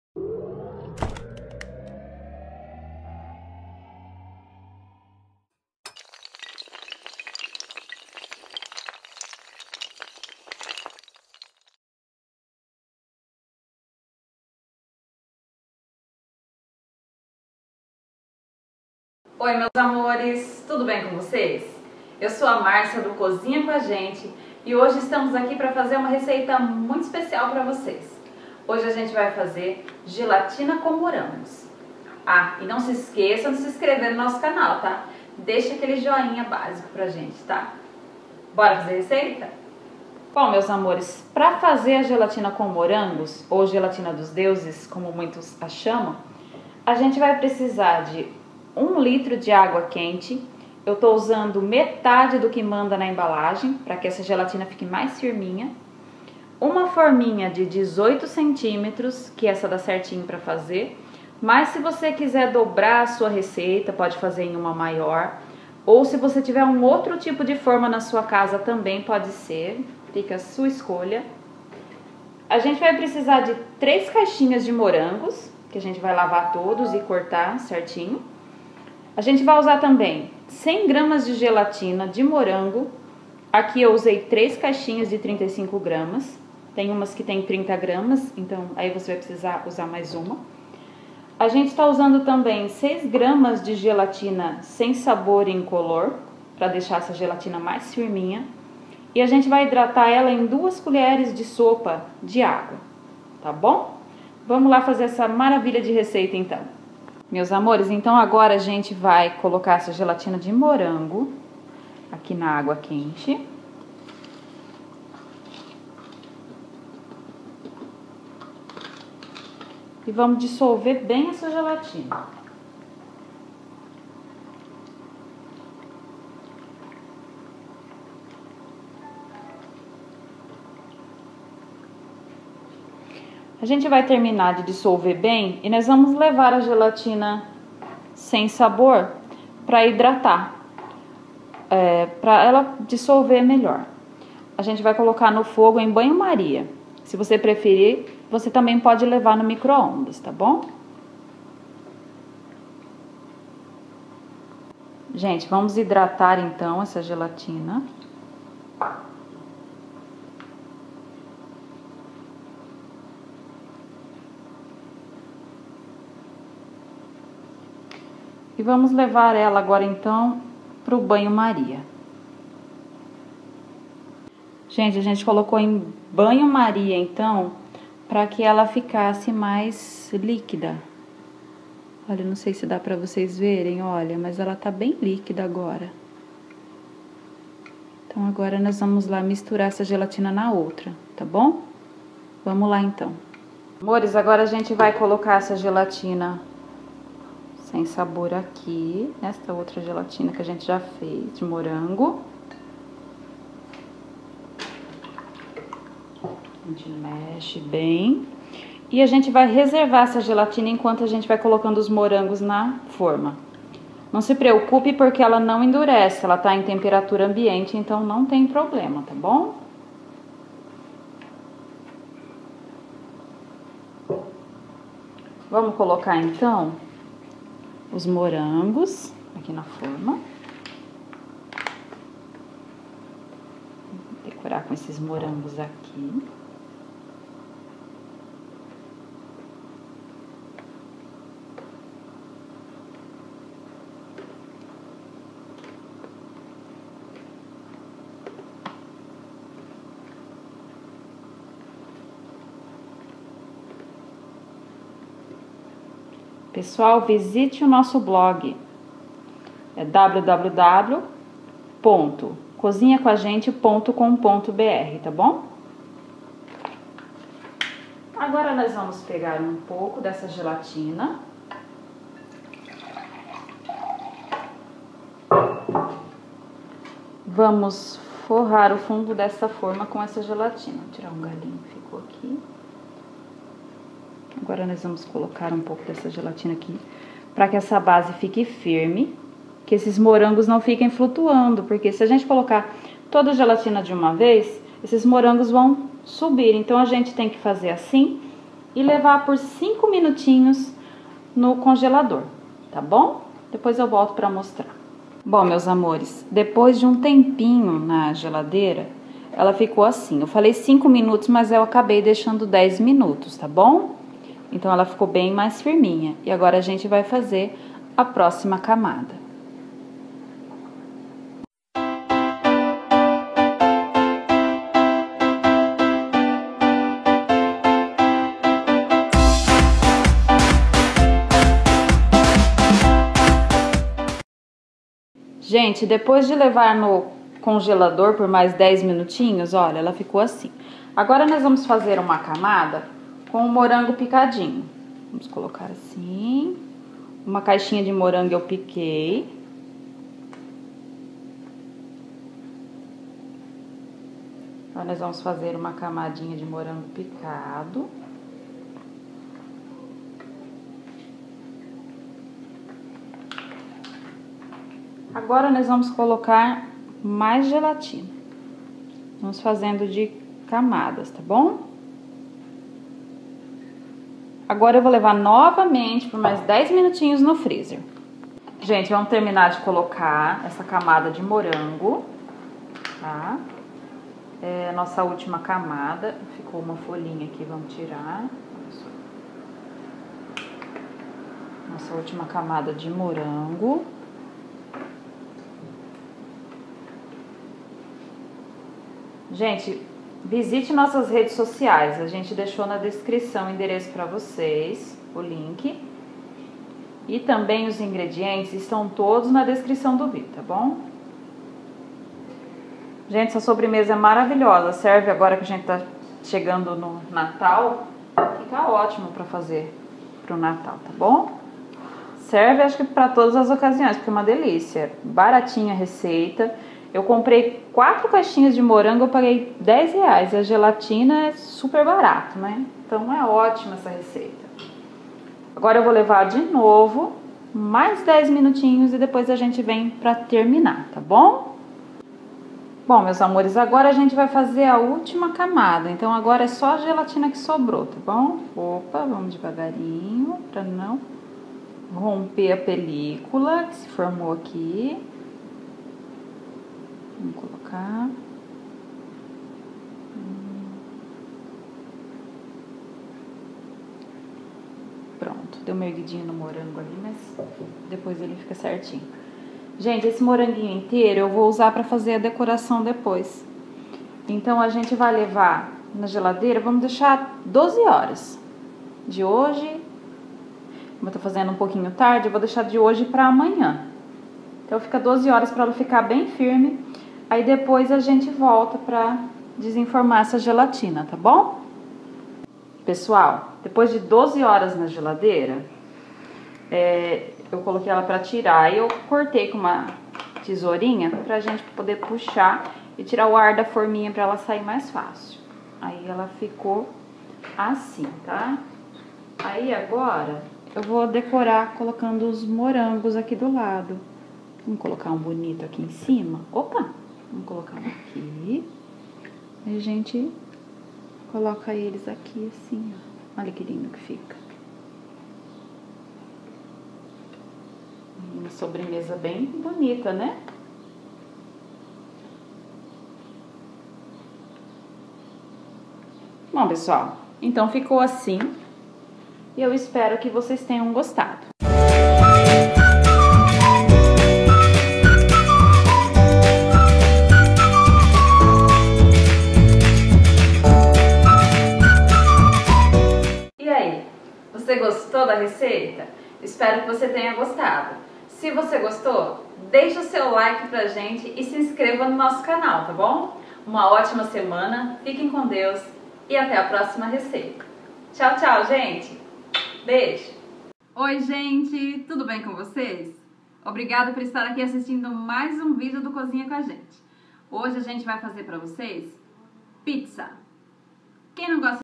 🔴GELATINA COM MORANGOS (GELATINA DOS DEUSES)#29
CURSO DE CUPCAKE MARAVILHOSO SE QUISER SABER MAIS ACESSE O
GELATINA COM MORANGOS OU GELATINA DOS DEUSES INGREDIENTES:
1 LITRO DE ÁGUA QUENTE
6 GRAMAS DE GELATINA INCOLOR SEM SABOR
100 GRAMAS DE GELATINA SABOR MORANGO
3 BANDEJAS DE MORANGOS
1  FORMA DE 18 CM
modo de preparo assista os vídeos :)

BOLO SUPER FÁCIL E RÁPIDO DE FAZER !